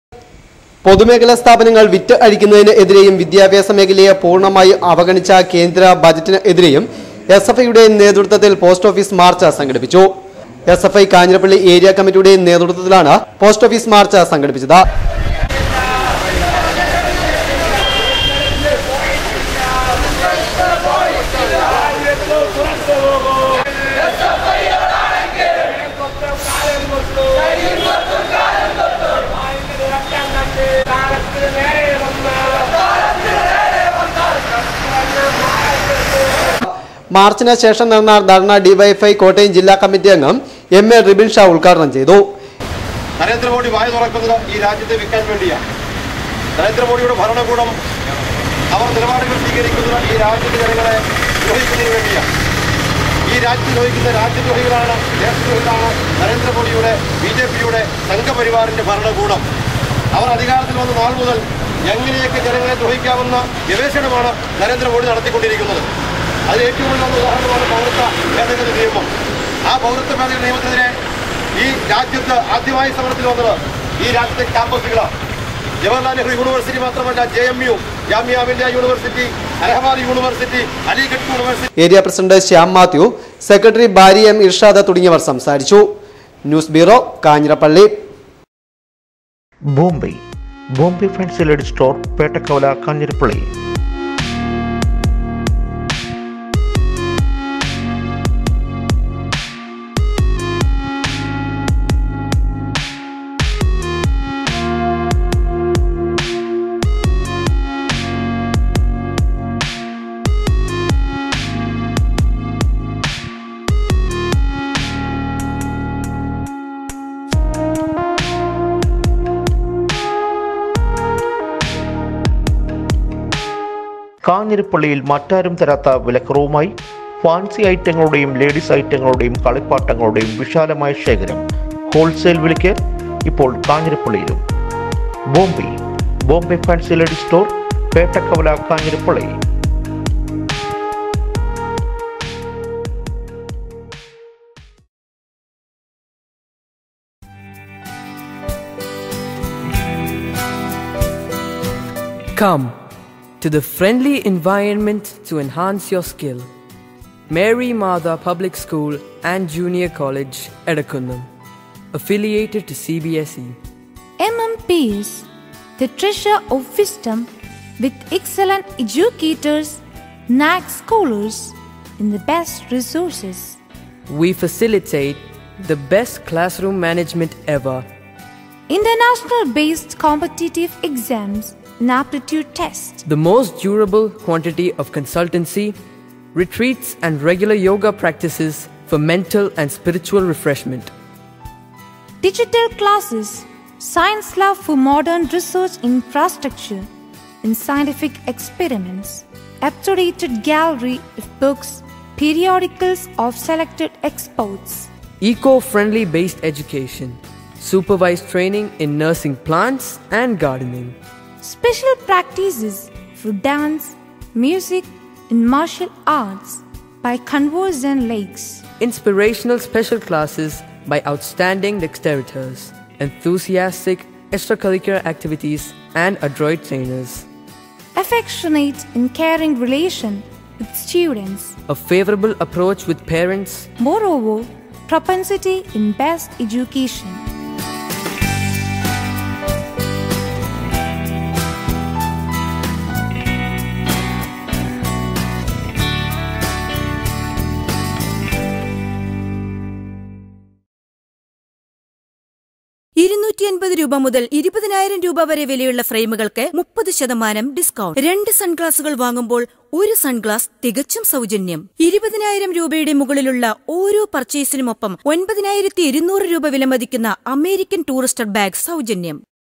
sfi march
വിദ്യാഭ്യാസ മേഖലയെ പൂർണ്ണമായി അവഗണിക്കുകയും, പൊതു മേ ഖലാ സ്ഥാപനങ്ങളെ വിറ്റ് തുലക്കുകയും ചെയ്യുന്ന കേന്ദ്ര ബജറ്റിനെതിരെ വിദ്യാർഥി പ്ര തിഷേധം